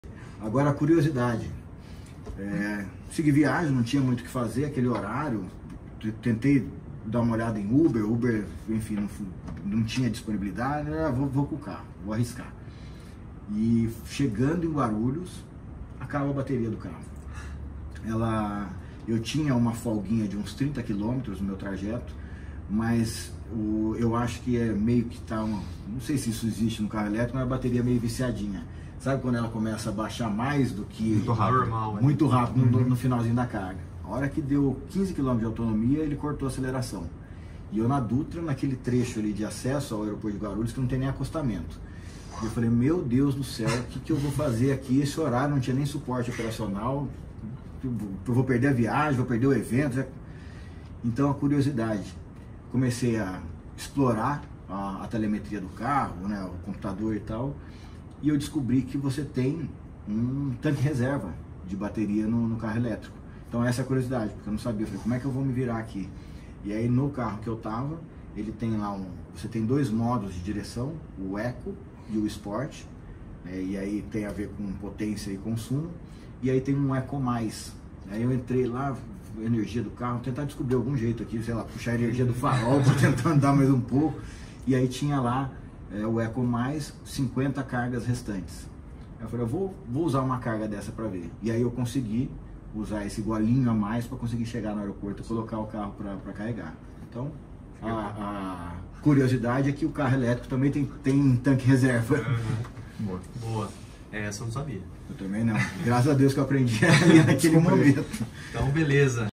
Reserva de baterias em carros elétricos
O CEO do Observatório Nacional de Segurança Viária do ONSV, Paulo Guimarães,
 fala sobre um dos elementos mais importantes do carro elétrico: a bateria!